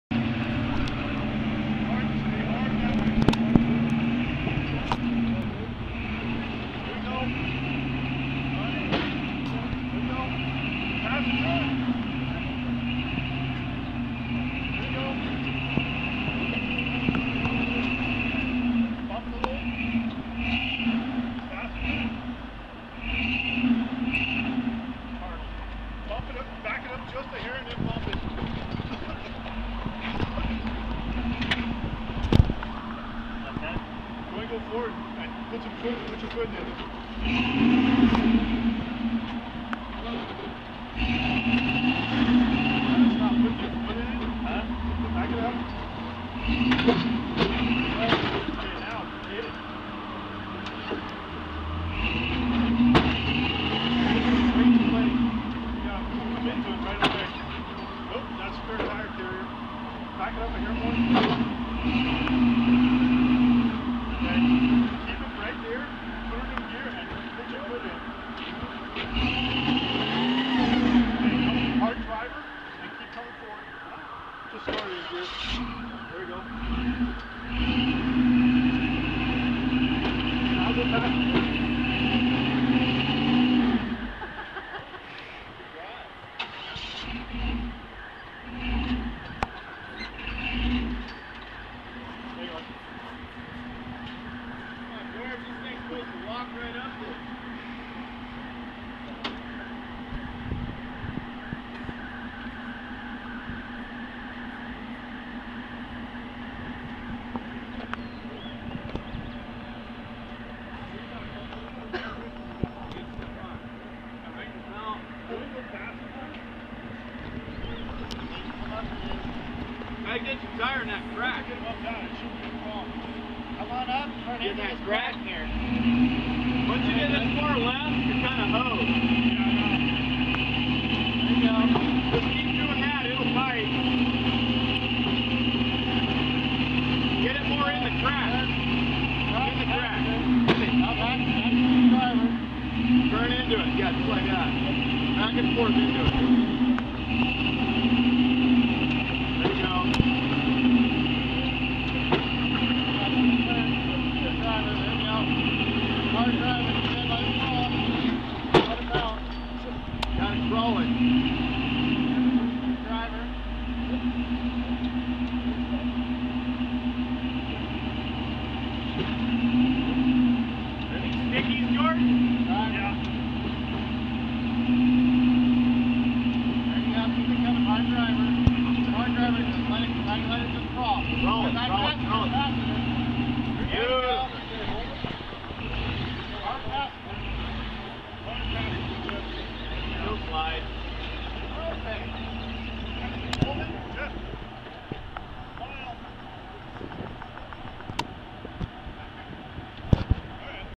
Jeff from Lone Star Cruisers in Soup Bowl
Jeff came out from Texas to Rubithon 2011 and George let him play in Soup Bowl.  I zoomed in to show the crack you need your front tire in to get up.